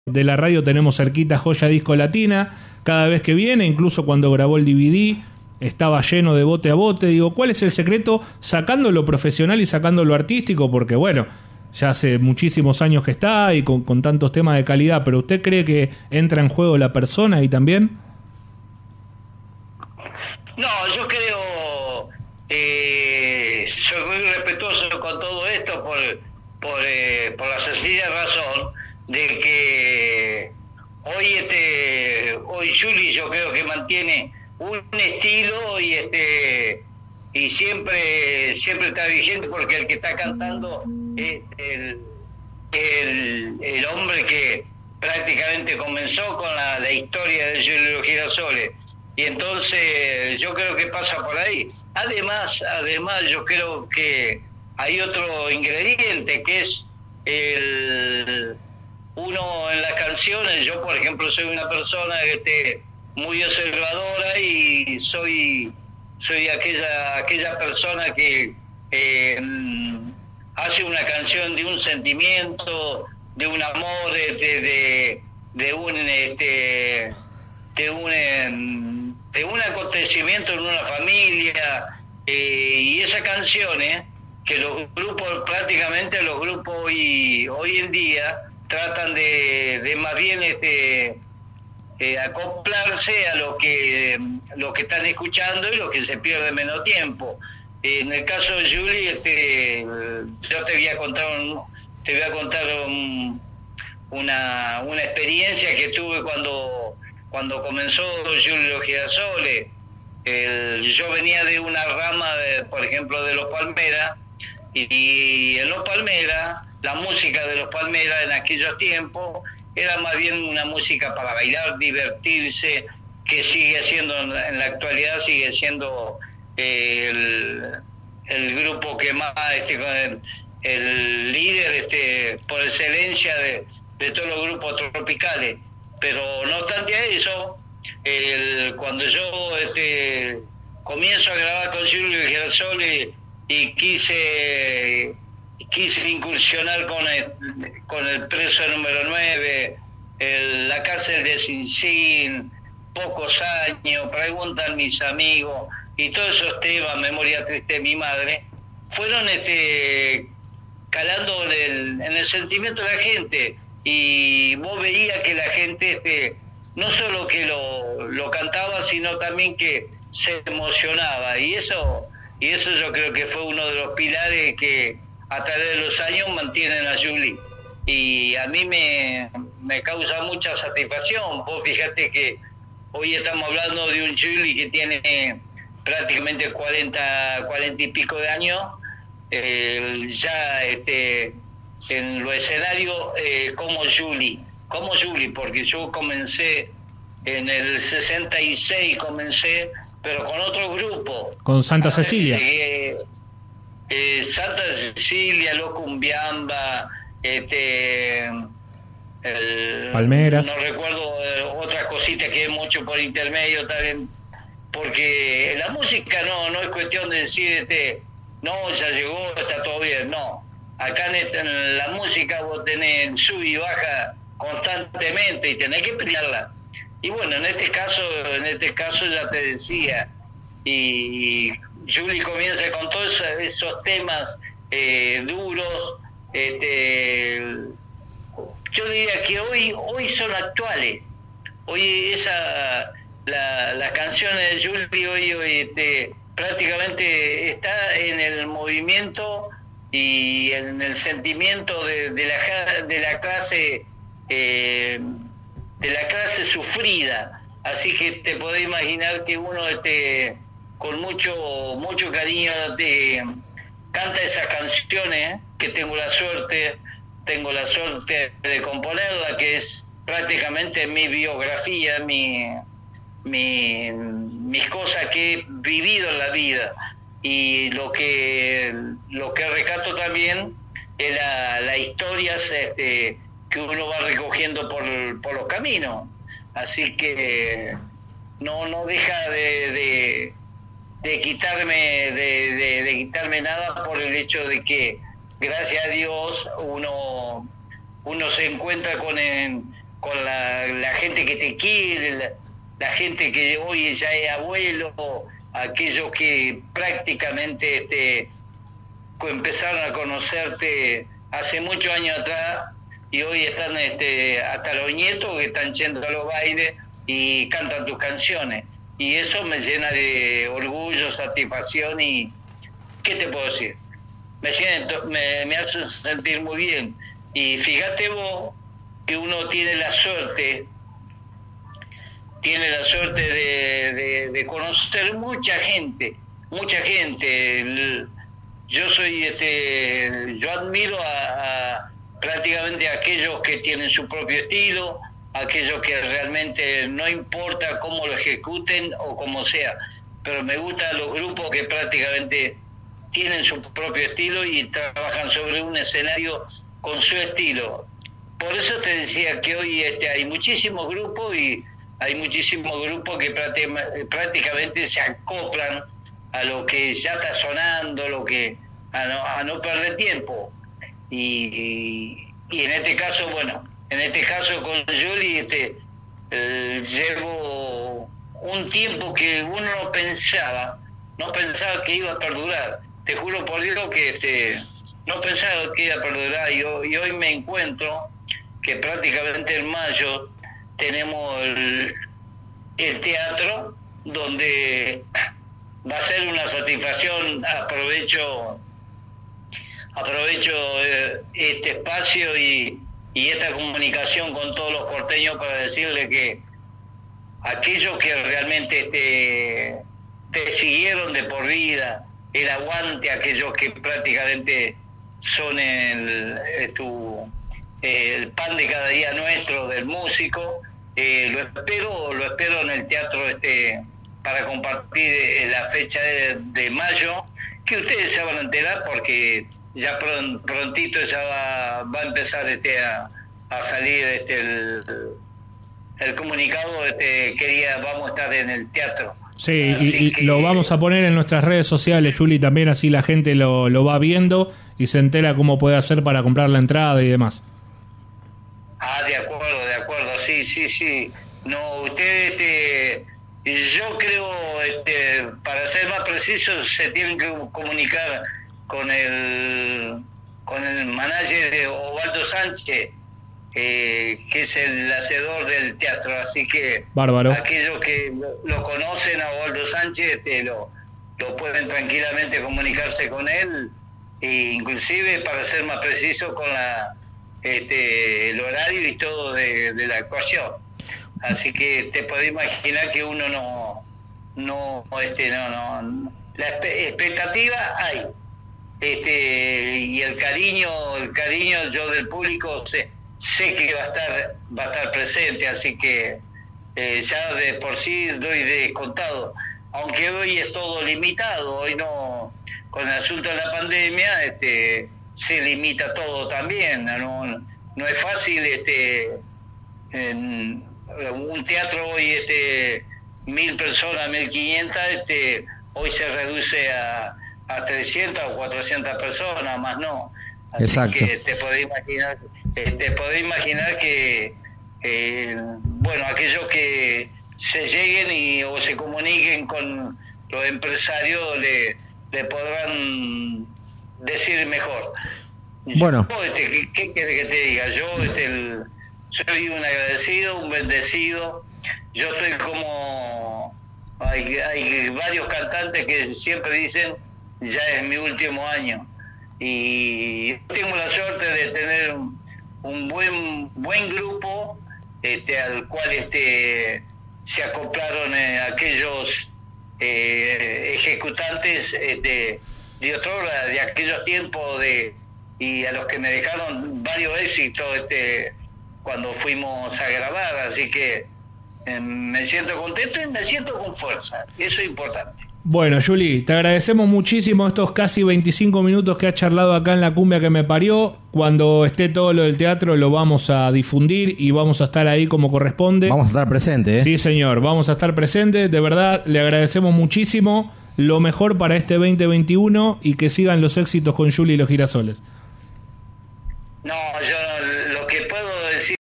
Entrevista a "Yuli" (Yuli y los Girasoles) - 4/2/21 - Parte II
Charlamos con el "Rey" sobre el gran presente de la banda, sus más de 45 años con la música, y muchos temas más. Ojalá les guste! Gracias!